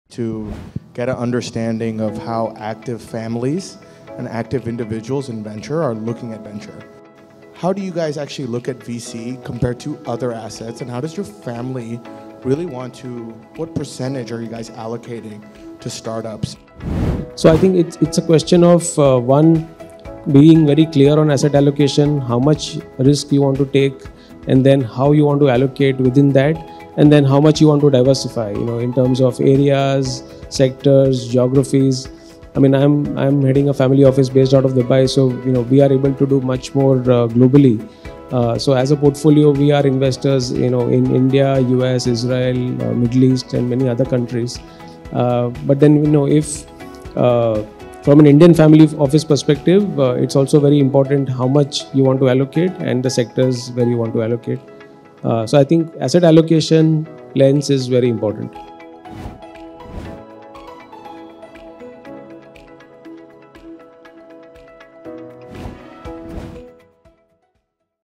Capital Allocation in Startups
Hershel Mehta, co founder of 2am VC is in talk with Chetan Mehta, CEO of WAMI Capital about capital allocation for startup investment.